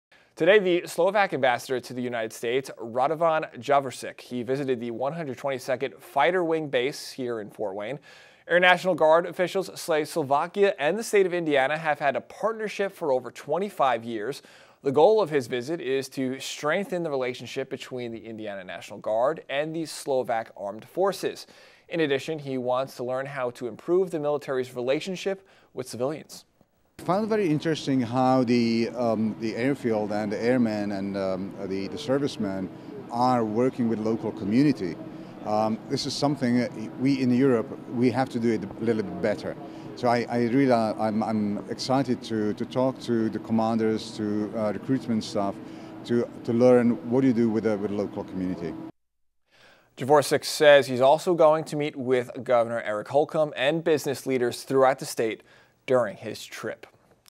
Slovak ambassador visits Fort Wayne air base, strengthen partnership between Indiana and Slovakia
On Tuesday the Slovak Ambassador to the United States, Radovan Javorcik, visited the 122nd Fighter Wing base in Fort Wayne.  Air National Guard officials say Slovakia and the state of Indiana have had a partnership for over 25 years through the State Partnership Program.